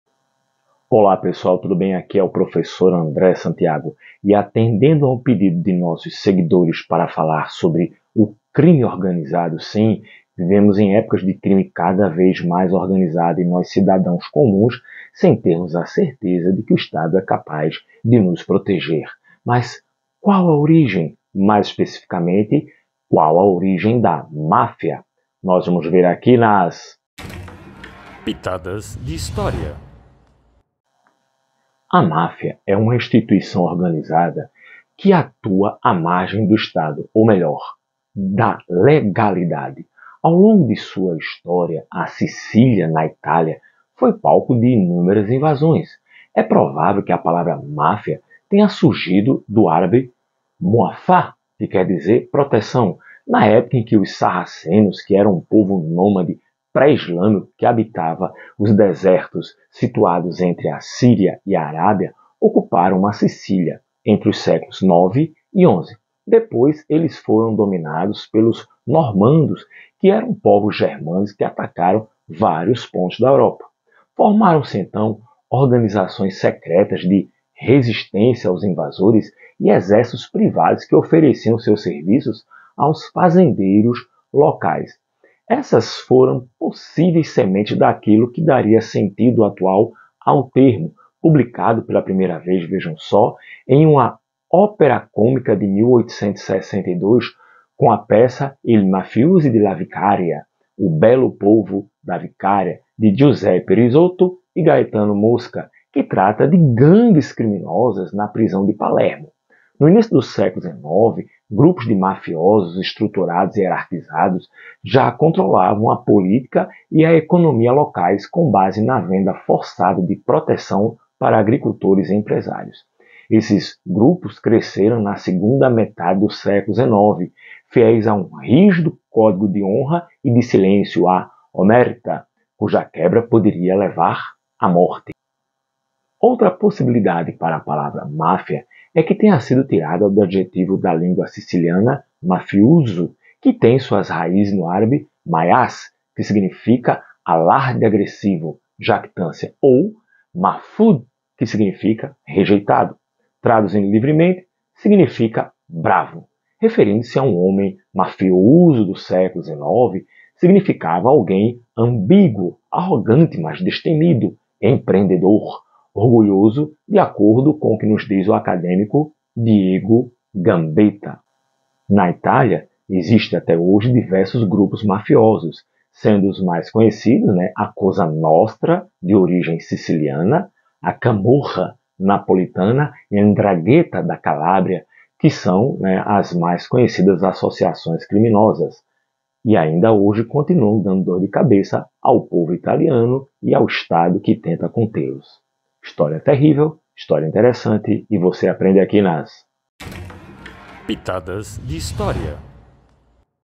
A origem da máfia - Pitadas de História Episódio 98
Origem histórica da máfia e suas relações com as invasões sarracenas e normandas na Idade Média